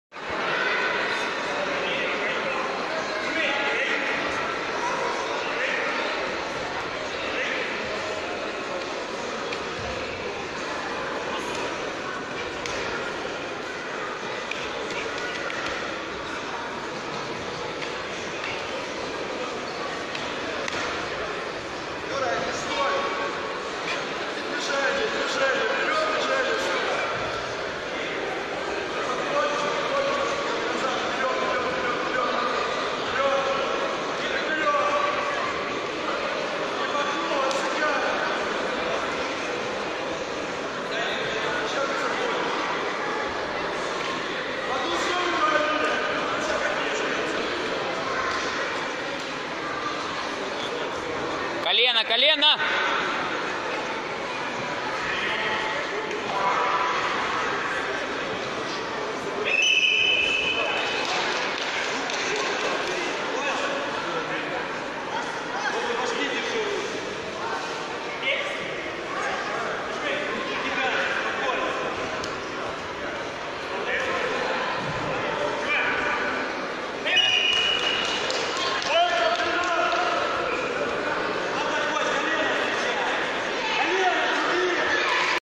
Абсолютный ЧУ 2017 - Финал Лободюк Юрий - Гишко Андрей (ака)
30 сентября 2017г в Киеве прошел абсолютный чемпионат Украины среди мужчин по киокушин карате (IKO)
абсолютный чемпионат Украины 2017
 Финал Лободюк Юрий - Гишко Андрей (ака)